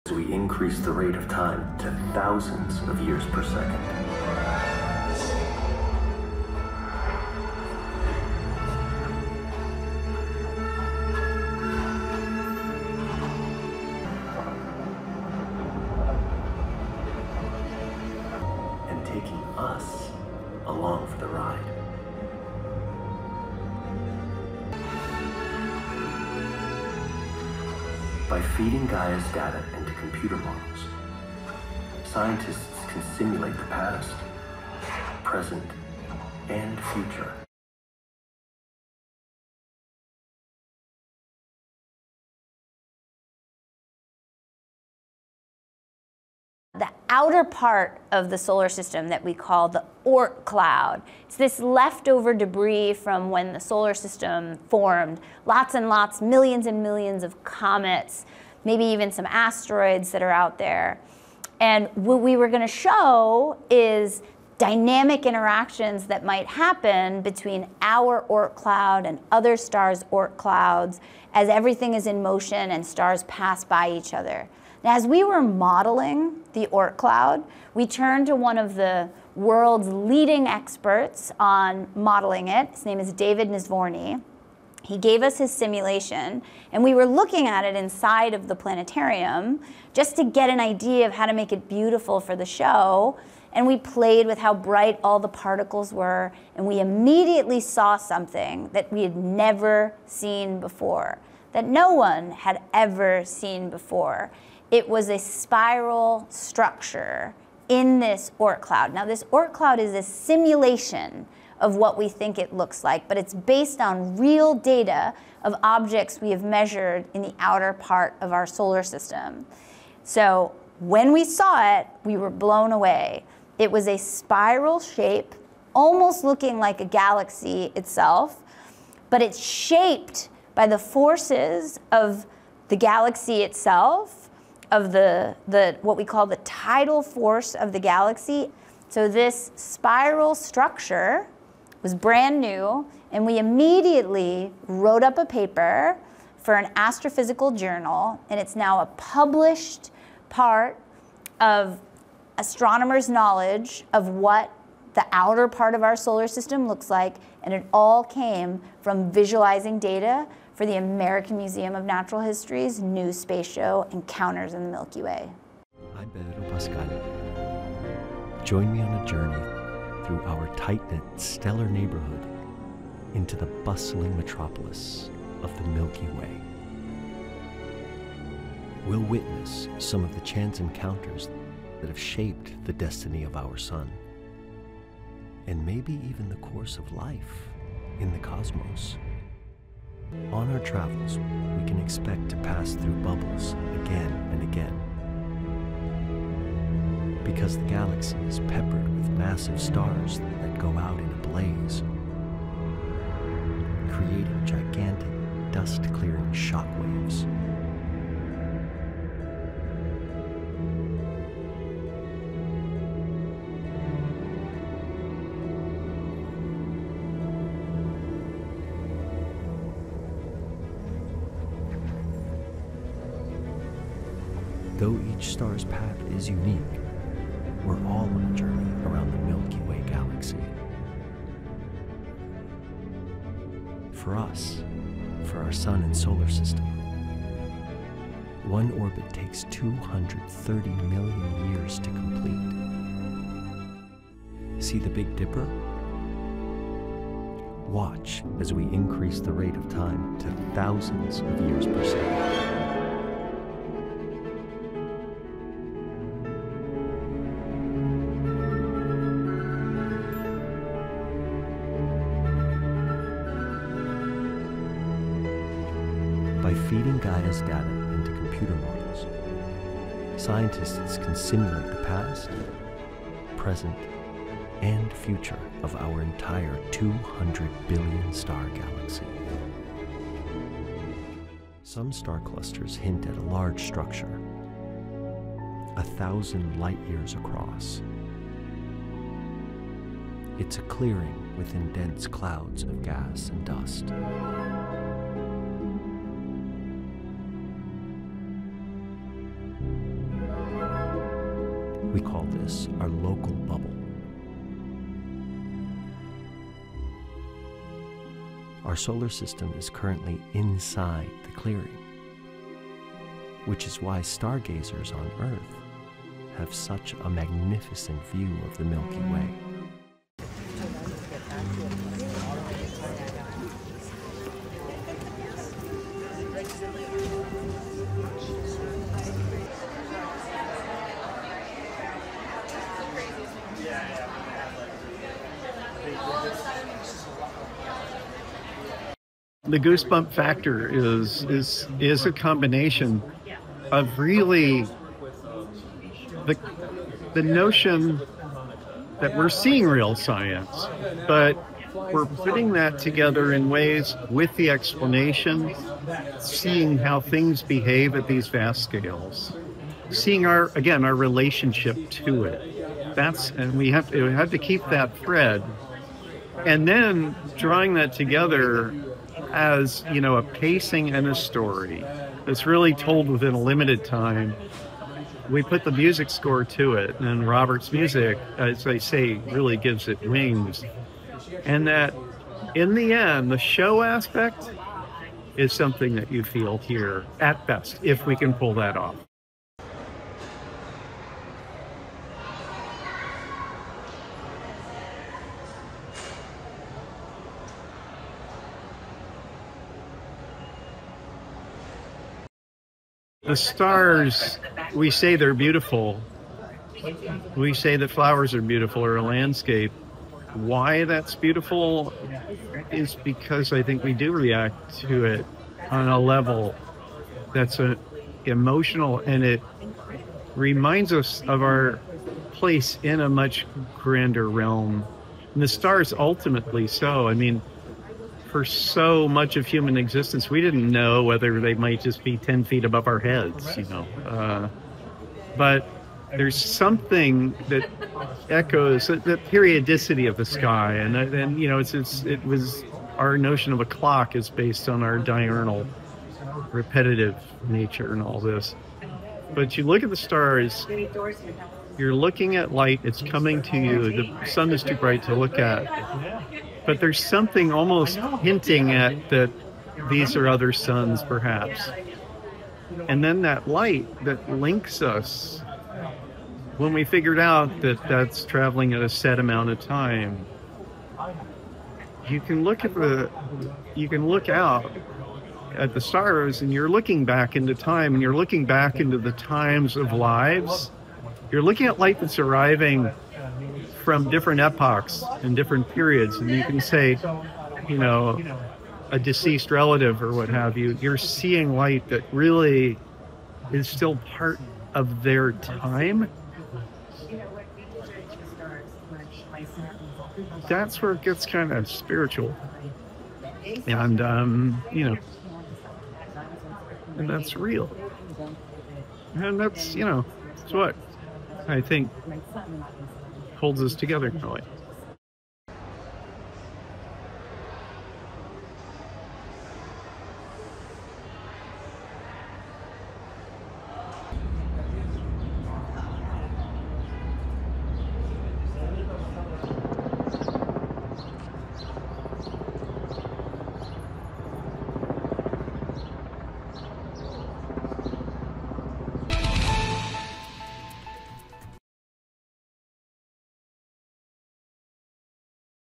Hayden Planetarium’s “Encounters in the Milky Way” Reveals Oort Cloud Spiral | News9
The American Museum of Natural History’s Hayden Planetarium debuts “Encounters in the Milky Way” on June 9, a visually stunning show exploring the solar system’s journey through the galaxy. Featuring groundbreaking simulations including a newly discovered spiral structure in the distant Oort cloud, the show combines scientific data, narration by Pedro Pascal, and immersive visuals to deepen understanding of our cosmic surroundings and humanity’s connection to the stars.